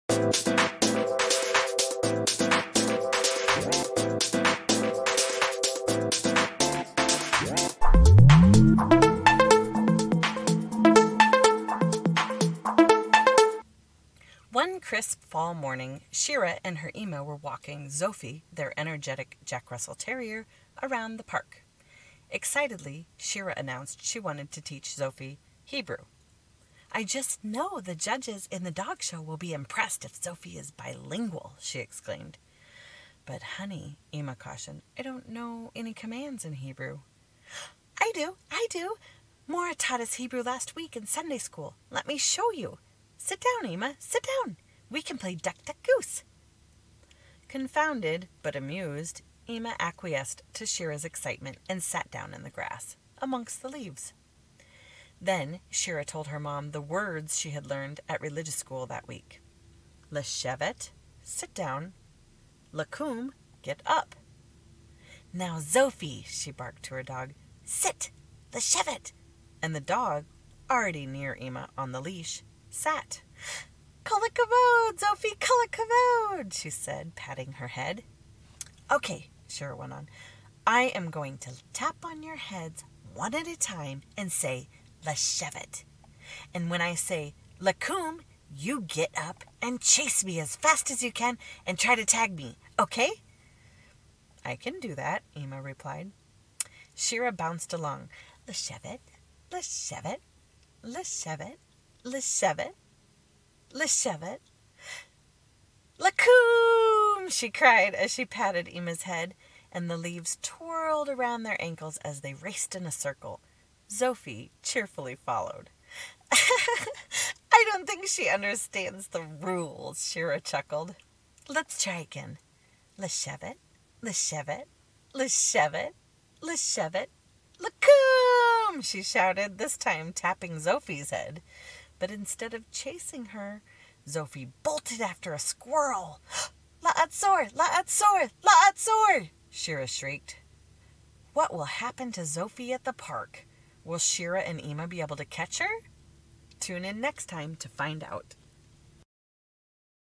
The Adventures of Shira and Tzofi part 1 (Foundations, Level 1)
Enjoy this short Hebrew Review video for Foundations, Level 1 students using the free curriculum
Shira, Ima and Tzofi play Duck, Duck, Goose using Hebrew at the park, but Tzofi doesn't really "get it..."